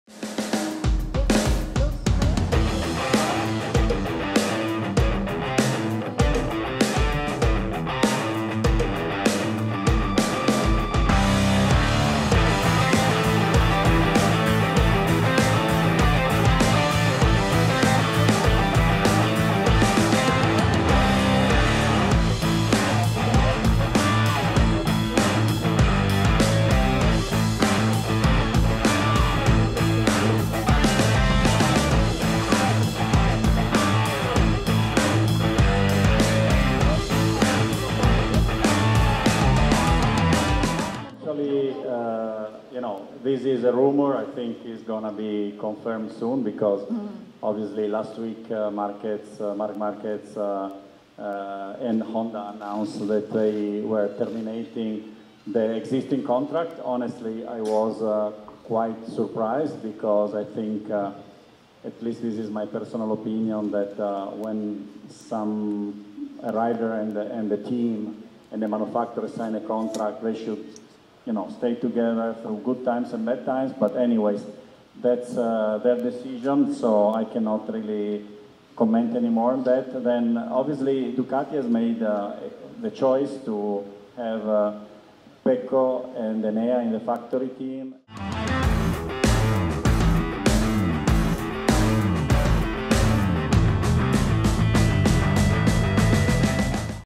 Paolo Ciabatti Respons Rumor Marc Marquez ke Ducati
Direktur Olahraga Ducati, Paolo Ciabatti enggan menanggapi kepindahan mantan juara dunia MotoGP 6 kali asal Spanyol, Marc Marquez.

Produsen motor asal Bologna Italia ini menghormati kontrak Marquez.

Ia memastikan ducati ditunggangi juara bertahan Pecco Bagnaia dan Enea Bastianini di ajang MotoGP tahun ini.